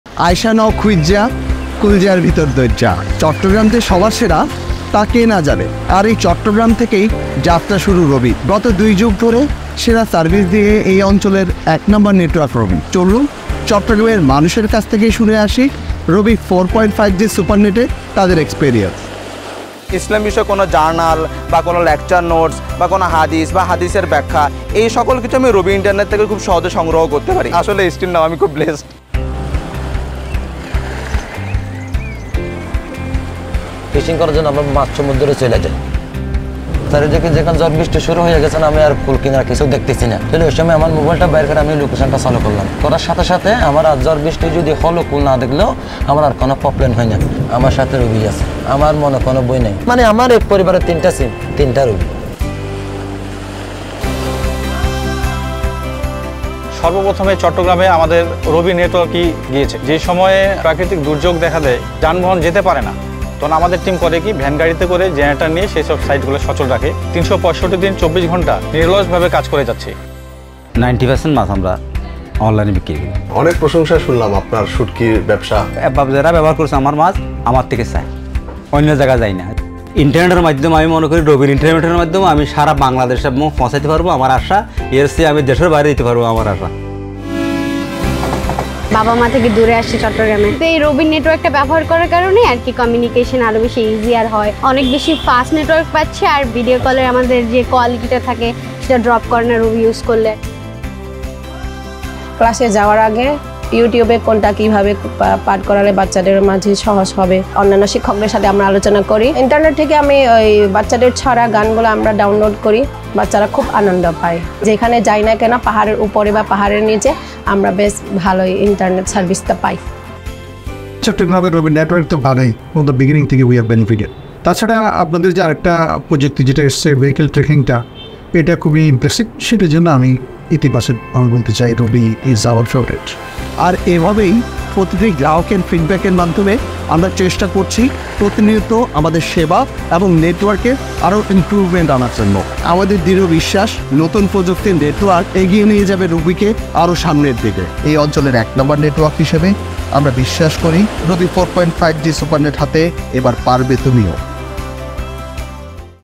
চট্টগ্রামজুড়ে রবি 4.5G Supernet
বন্দরনগরী চট্টগ্রাম থেকে দুই যুগেরও বেশি সময় আগে শুরু হয়েছিল রবি’র পথচলা। আজ রবি’ই চট্টগ্রামের সবচেয়ে শক্তিশালী নেটওয়ার্ক। চলুন, শুনে নেয়া যাক 4.5G সুপারনেট হাতে চট্টগ্রামবাসীর সাফল্যের গল্প।